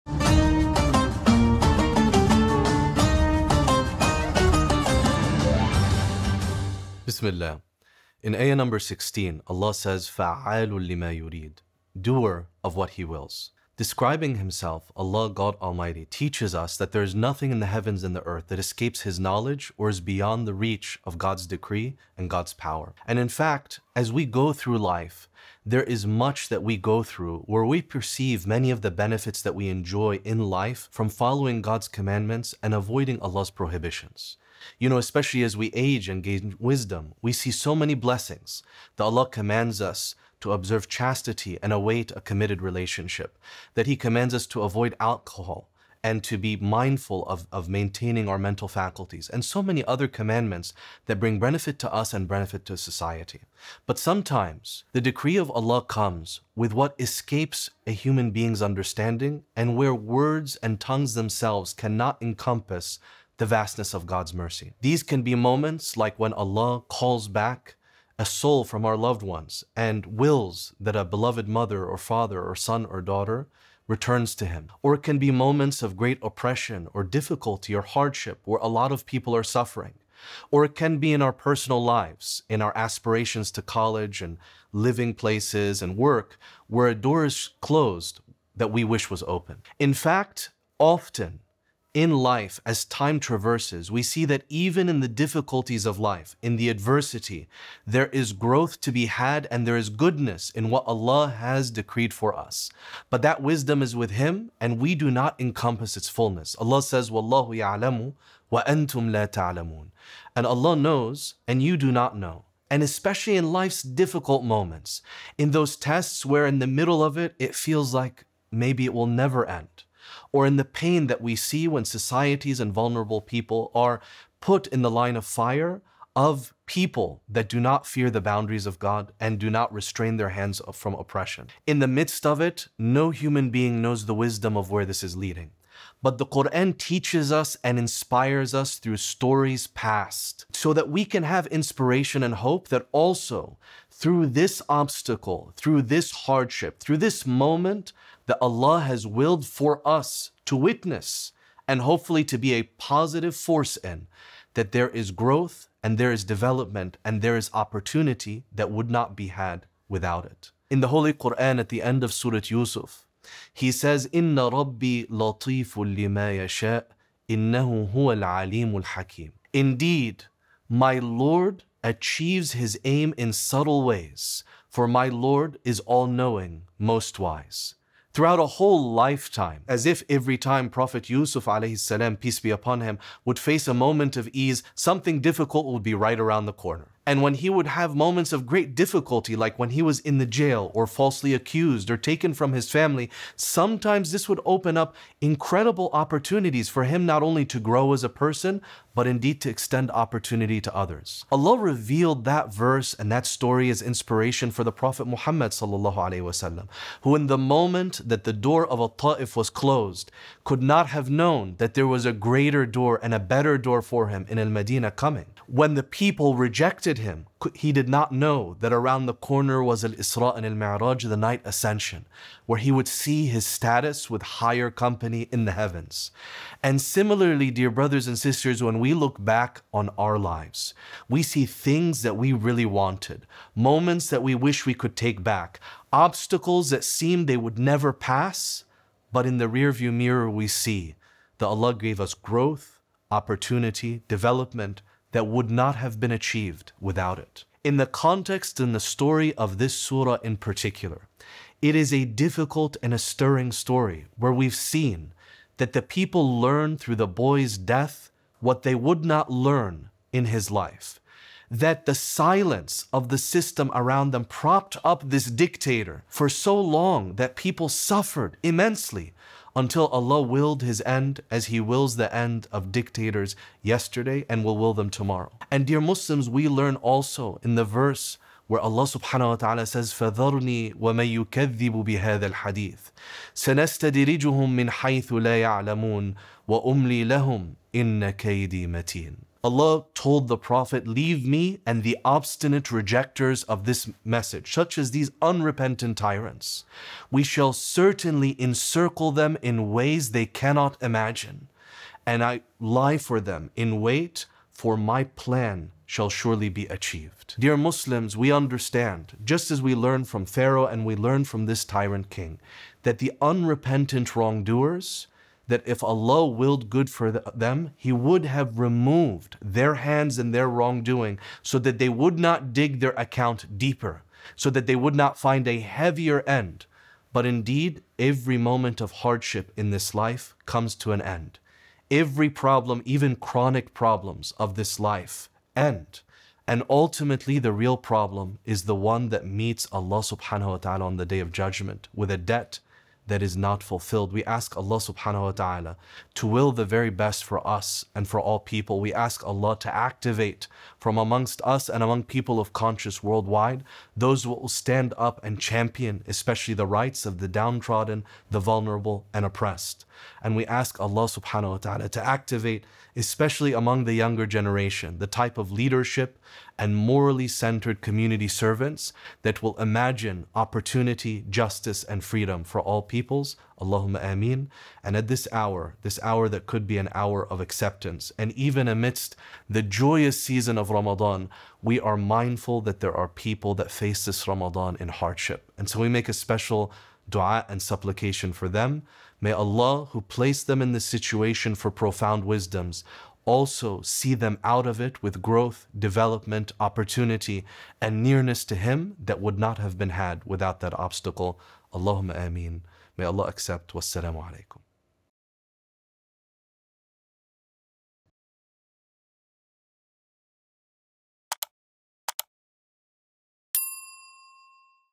Ep 25: The Doer of What He Wills, Dr. Mohamed AbuTaleb | In The Shade of Ramadan
Dr. Mohamed AbuTaleb joins us again to reflect on ayah 16 of Surah al-Buruj. As we feel overwhelmed by tyrants and corrupt authority figures, Allah reminds the believers that it is only Him who has ultimate control and can do whatever He wants.

Engage with the Surah even deeper.

Sign up to receive episodes in your inbox